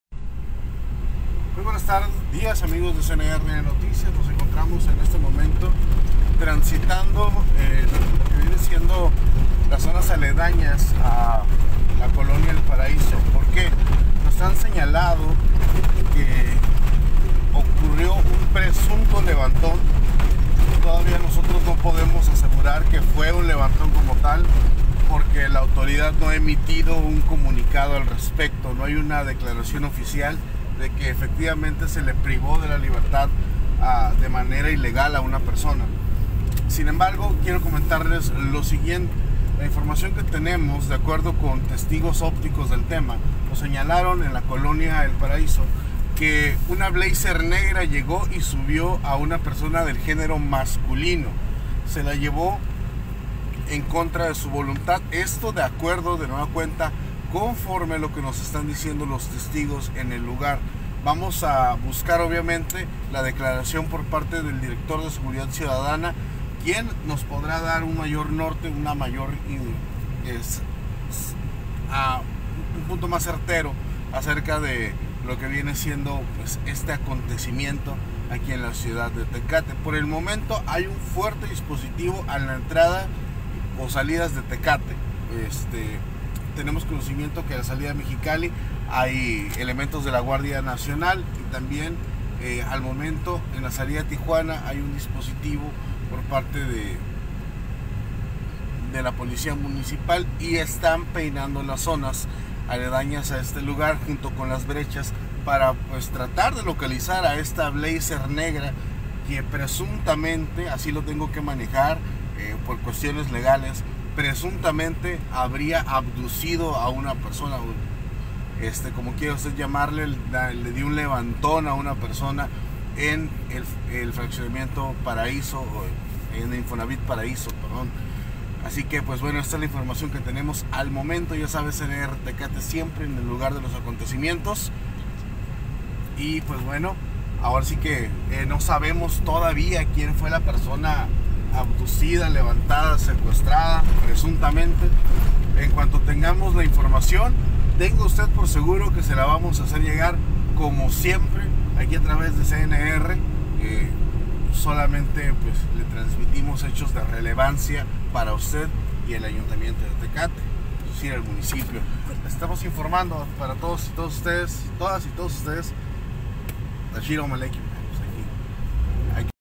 CNR Tecate Presunto levantón en el paraíso
Una llamada realizada esta mañana en la colonia El paraíso señalaba que una persona había sido abducida o subida contra su voluntad a un automóvil tipo blazer color negro, los hechos señalaron ocurrieron en la colonia El paraíso en la ciudad de Tecate, a pesar de no contar con declaración oficial unidades de la Policía Municipal y fiscalía peinaron la zona asimismo se montaron algunos retenes lo anterior y de acuerdo con lo señalado extraoficialmente sin resultados positivos